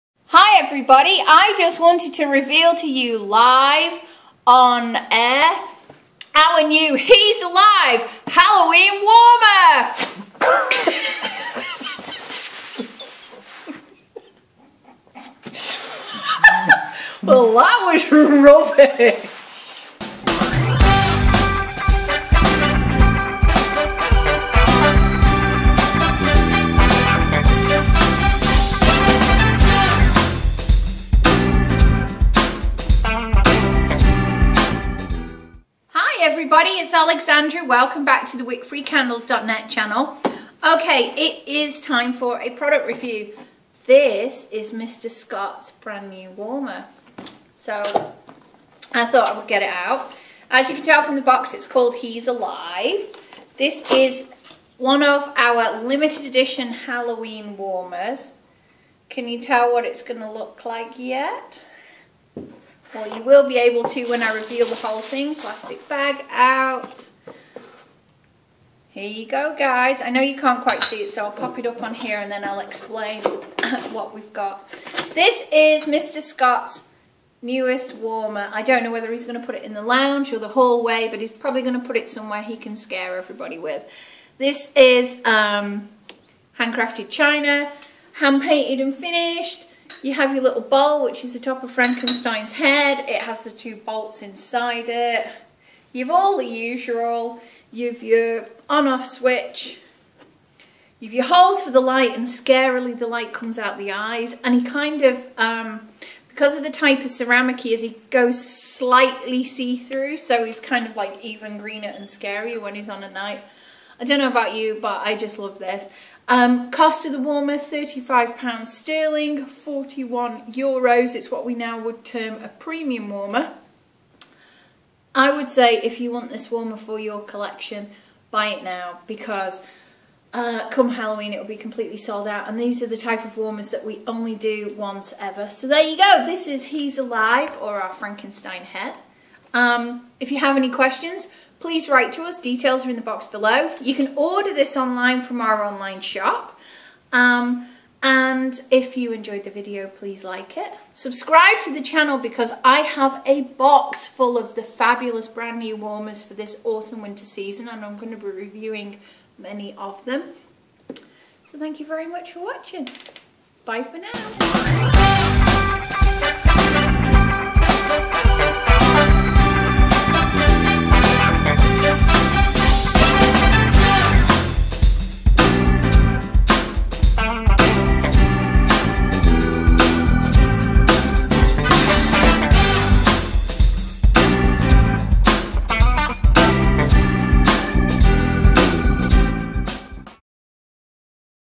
Scented Candle Product Review - Scentsy He's Alive Warmer - Halloween Scented Candles
Buy this Warmer (change the flag for the country you wish to send too)

Scented Candle Product Review - Scentsy He's Alive Warmer

I just love this limited edition Scentsy warmer. Hes ALIVE! Perfect for boys, men and Halloween check out my scented candle product review on this awesome warmer.